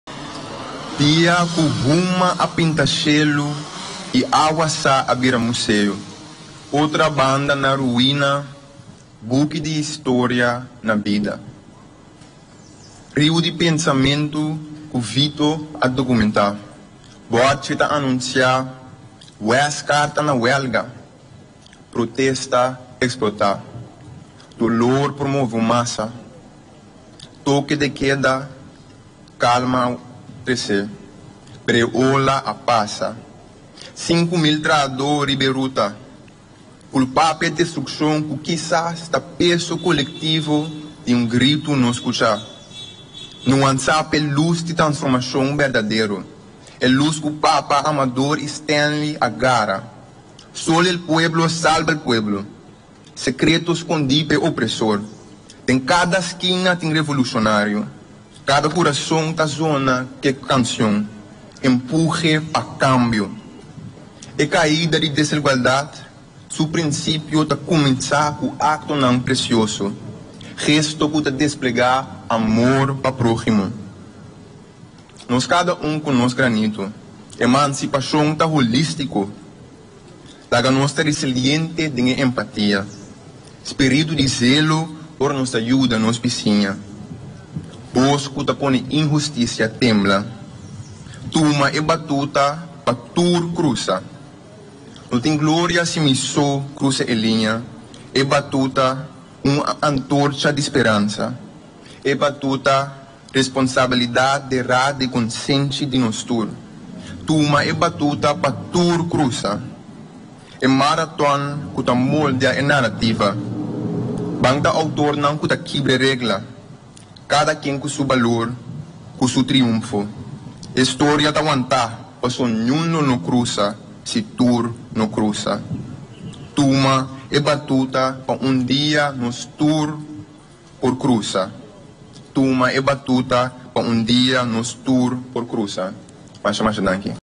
Tuma e batuta (30 di mei)
Performance in the heart of Punda, Curçao.
At the monument of Wilson “Papa” Codett.
Commemorating the labor uprising of
may the 30th - 1968.

Skirbí pa Ralph Winedt

Dia ku huma a pinta shelu
I awasa a bira museo
Otrabanda na ruina
Buki di Historia na bida

Riu di pensamentu ku Vitó a dokumentá
Bòrchi ta anunsia “Wescar ta na wèlga”
Protesta eksplotá
Dolor por move masa

Toke de keda, kalmu imponé..
Pero e ola a pasa.

Sinku mil trahado rib’e ruta
Kulpá p`e destrukshon
ku  kisas ta peso kolektivo
di un gritu no skuchá.

Nuansá p’e lus di transformashon berdadero.
E lus ku Papa, Amador i Stanley a gara:
“Solo el pueblo salva al pueblo”
Sekreto skondí p’e opresor:
Den kada skina tin revolushonario
Kada kurason
 ta zona k`e kansion:
“Empuhe pa kambio”.

E kaída di desigualdat,
su prinsipio ta kuminsá ku aktonan presioso
Gesto ku ta desplegá “amor pa próhimo”.

Nos kada un ku nos granito
Emansipashon ta holístiko.

Laga nos ta resilente den empatia
Spiritu di zelo ora di yuda nos bisiña
Bos ku ta pone inhustisia tembla

Tuma e batuta
pa tur krusa

No tin gloria si mi so  krusa e liña.
E batuta, un antorcha di speransa
E batuta, responsabilidat derá den konsenshi di nos tur.

Tuma e batuta
pa tur krusa

E maraton ku ta moldia e narativa
Ban ta outornan ku ta kibra regla
Kada personahe ku su balor, ku su triumfo
E storia ta wantá, paso niun lo krusa si tur no  krusa.

Tuma e batuta pa un dia
nos tur
por krusa.